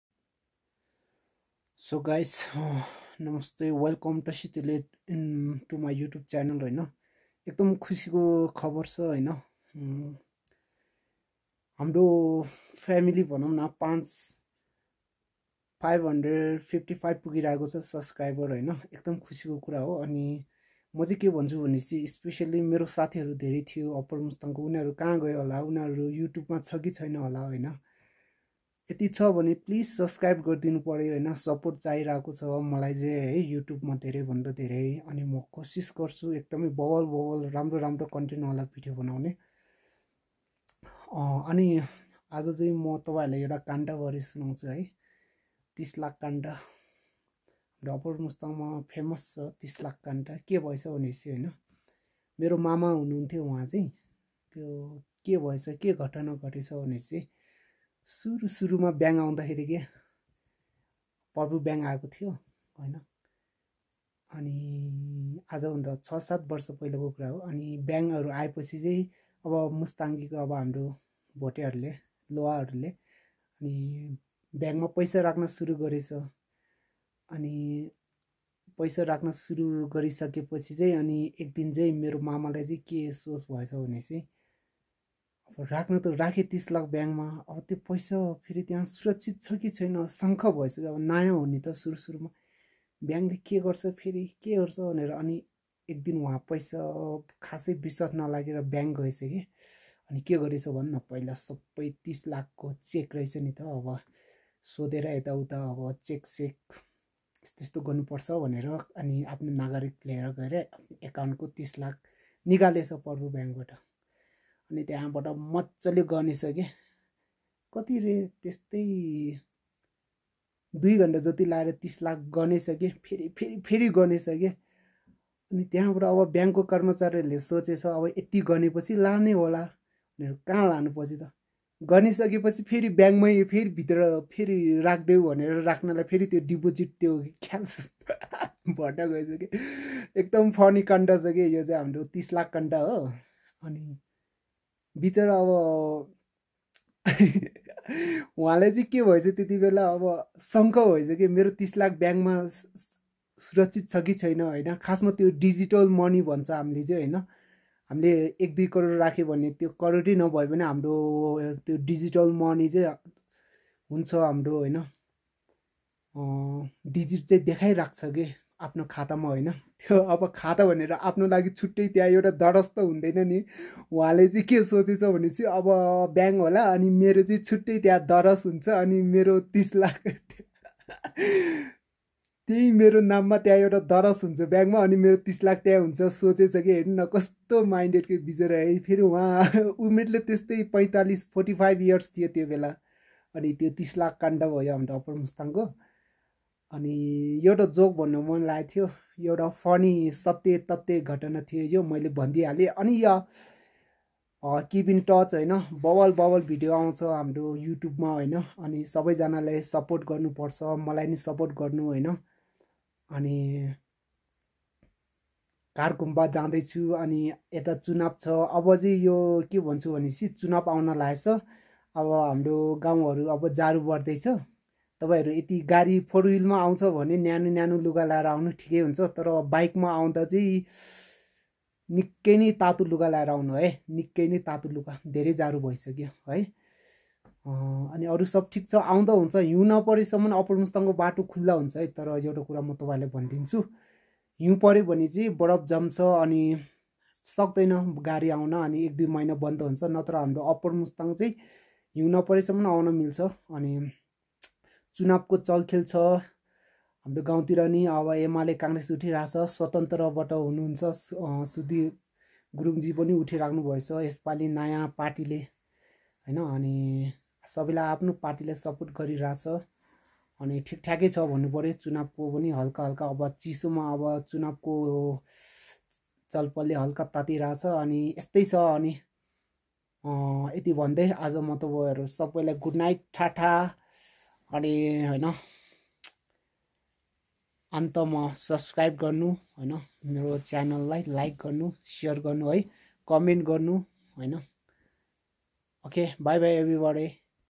Uppermustang ko 30 lakh kanda. This is what happen when we don't trust Pharbu bank. Uppermustang.
Please support my YouTube channel. Ask me anything about Uppermustang on comment box.  Uppermustang latest update.